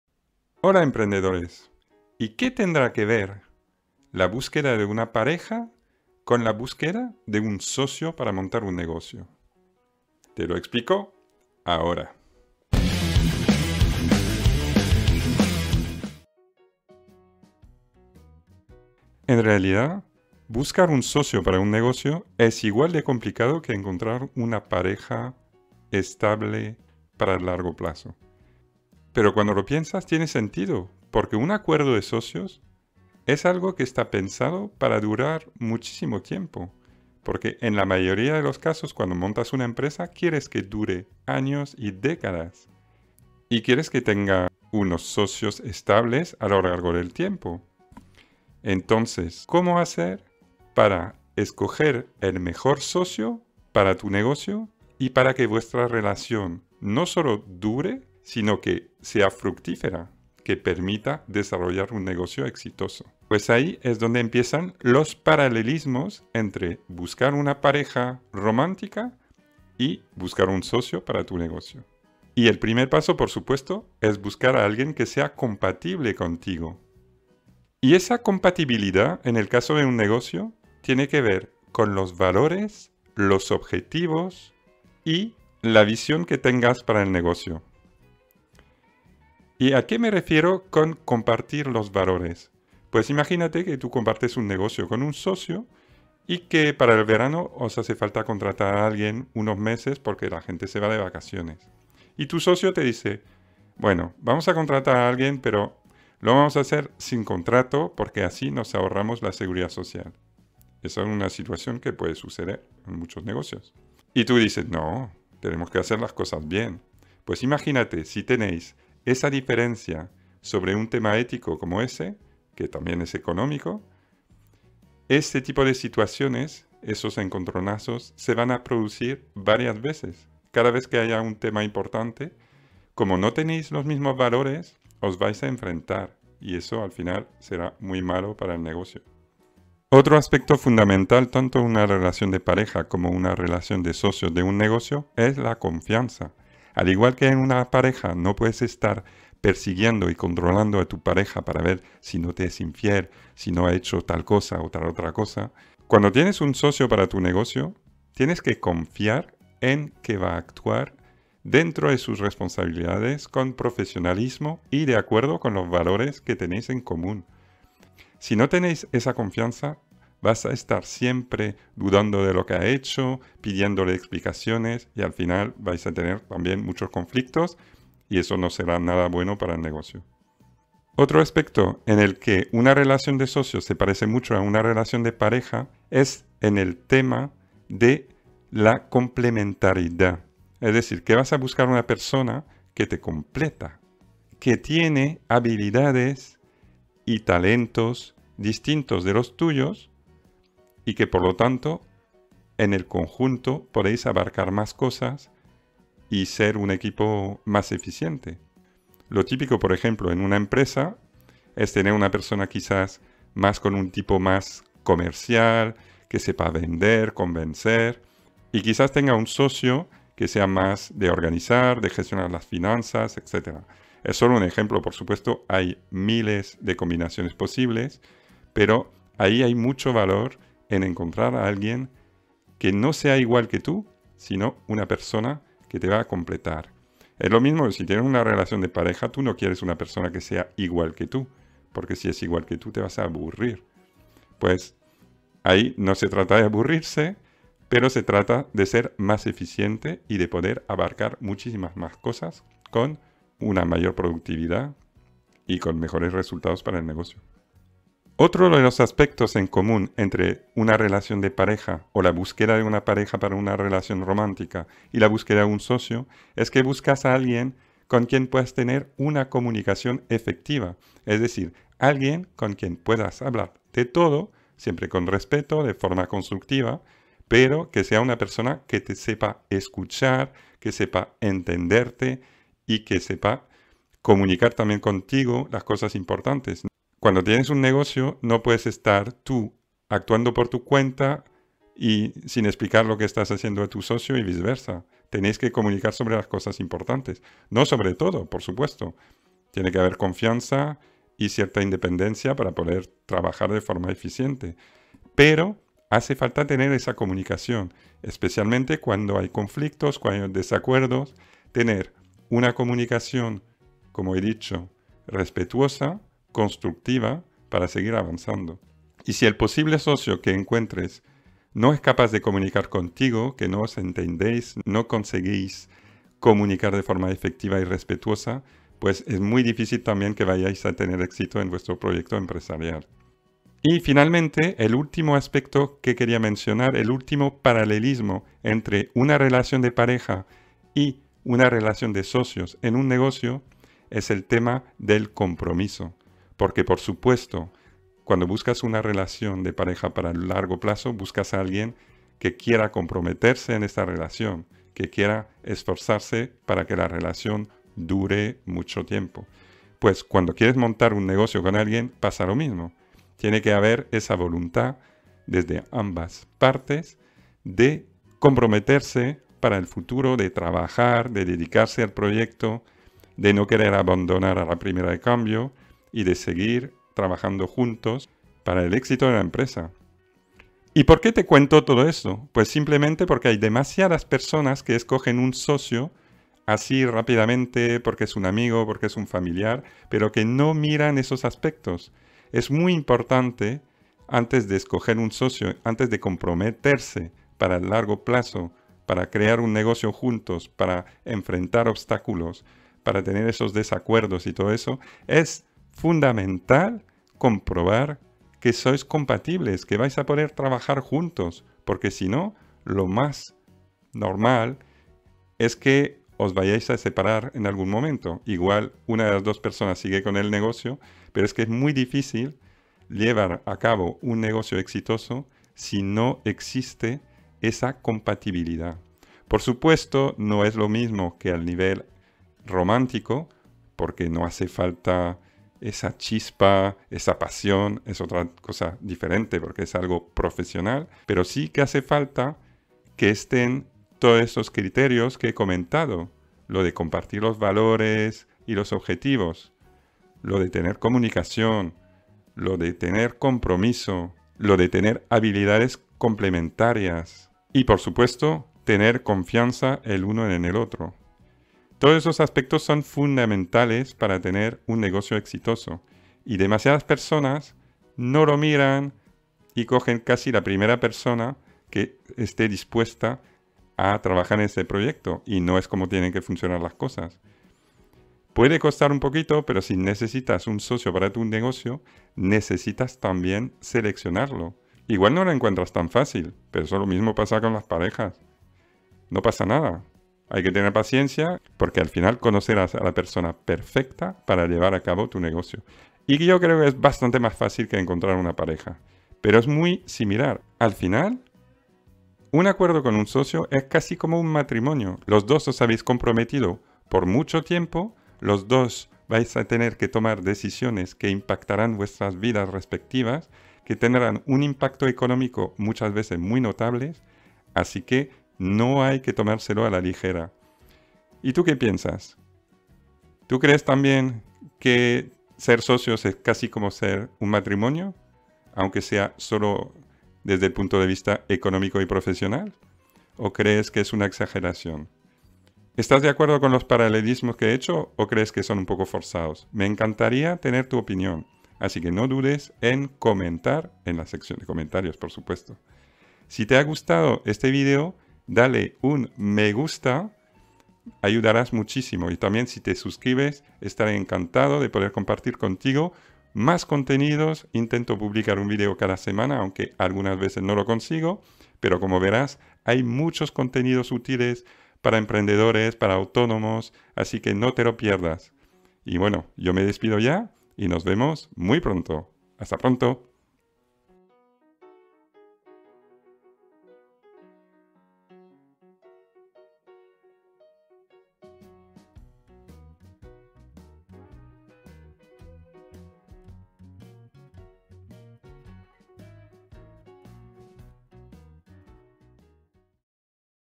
Buscar socio es como buscar pareja
Encontrar un socio (o una socia) para tu negocio es muy parecido a buscar pareja. Te cuento alguno de los paralelismos entre esas dos búsquedas.

En el blog comparto cada semana consejos de gestión, ideas de negocio y muchos otros tips para emprendedores, no te los pierdas.

Capítulos:
00:00 Intro
01:11 Compatibilidad
02:19 Confianza
03:00 Complementariedad
04:17 Comunicación
05:33 Compromiso
08:41 Tu opinión